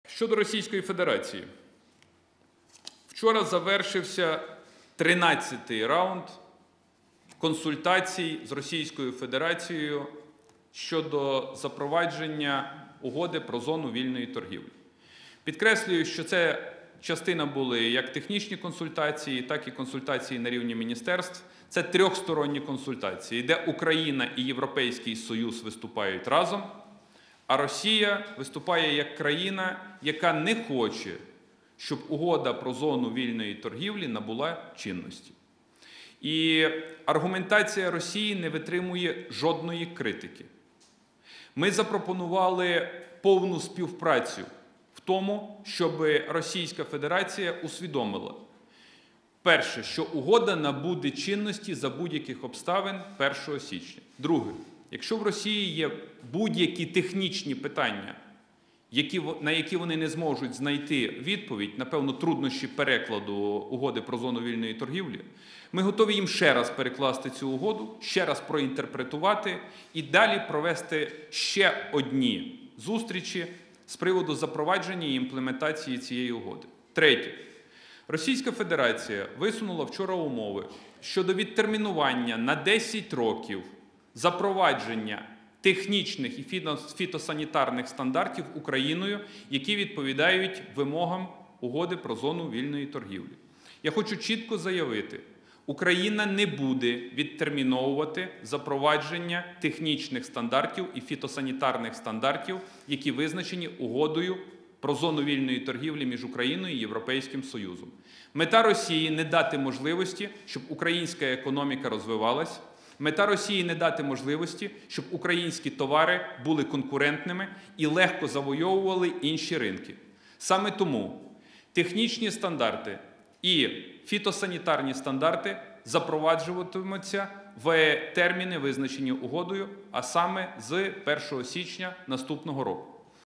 Україна не відтерміновуватиме запровадження технічних і фітосанітарних стандартів, визначених Угодою
«Україна не буде відтерміновувати запровадження технічних і фітосанітарних стандартів, які визначені Угодою про зону вільної торгівлі між Україною і Європейським Союзом», - заявив Прем’єр-міністр України, лідер партії «Народний фронт» Арсеній Яценюк на засіданні Уряду в середу, 2 грудня.

Арсеній Яценюк повідомив, що 1 грудня завершився 13-й раунд технічних консультацій і консультації на рівні міністерств щодо запровадження Угоди про зону вільної торгівлі між Україною та ЄС: «Це трьохсторонні консультації, де Україна і Європейський Союз виступають разом, а Росія виступає як країна, яка не хоче, щоб Угода про зону вільної торгівлі набула чинності».

«Аргументація Росії не витримує жодної критики. Ми запропонували повну співпрацю в тому, щоби Російська Федерація усвідомила, що Угода набуде чинності 1 січня за будь-яких обставин. Якщо в Росії є будь-які технічні питання, на які вони не зможуть знайти відповідь, - напевно, через труднощі перекладу Угоди про зону вільної торгівлі, - ми готові їм ще раз перекласти цю Угоду, ще раз проінтерпретувати і далі провести зустрічі з приводу запровадження й імплементації цієї Угоди», - підкреслив він.

Арсеній Яценюк підкреслив, що під час учорашніх переговорів РФ висунула умови щодо відтермінування на 10 років запровадження Україною технічних і фітосанітарних стандартів, які відповідають вимогам Угоди про ЗВТ з Євросоюзом.

«Мета Росії – не дати можливості, щоб українська економіка розвивалася. Мета Росії – не дати можливості, щоб українські товари були конкурентними і легко завойовували інші ринки»,  - наголосив він.

«Хочу чітко заявити: Україна не буде відтерміновувати запровадження технічних стандартів і фітосанітарних стандартів, які визначені Угодою про зону вільної торгівлі між Україною і Європейським Союзом. Технічні і фітосанітарні стандарти запроваджуватимуться в терміни, визначені Угодою, а саме з 1 січня наступного року», - підкреслив Арсеній Яценюк.

Приєднуйся!